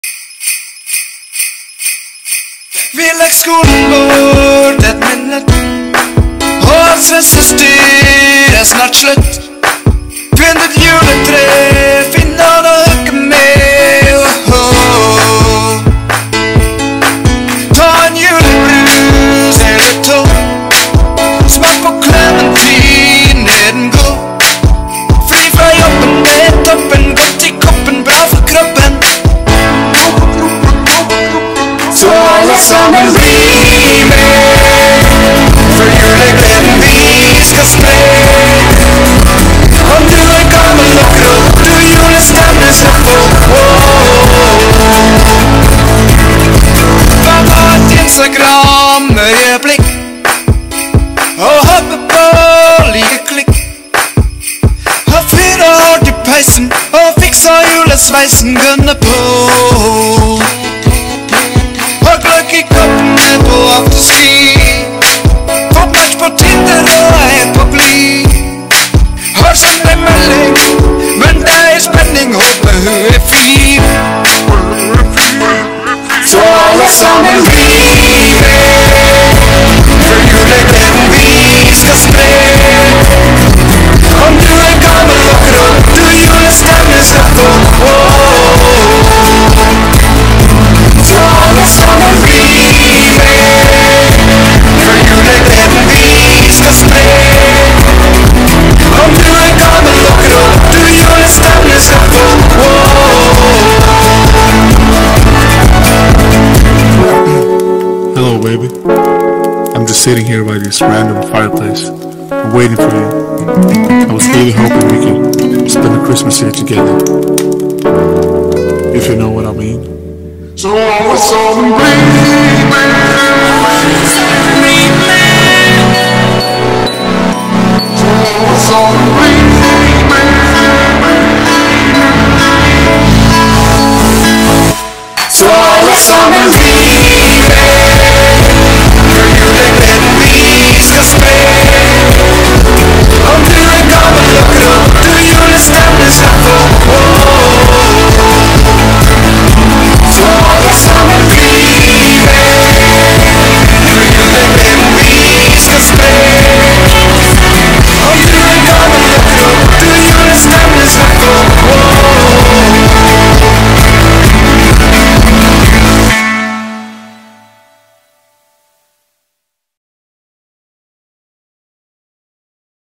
JULEGLEDE 2020🤶⭐️🎅🏻😷
Nissene på idrettsfag ved Fosen VGS ønsker alle en GOD JUL og GODT NYTT ÅR🎅🏻✨🧑🏻‍🎄 Husk å spre glede, ikke virus😃😷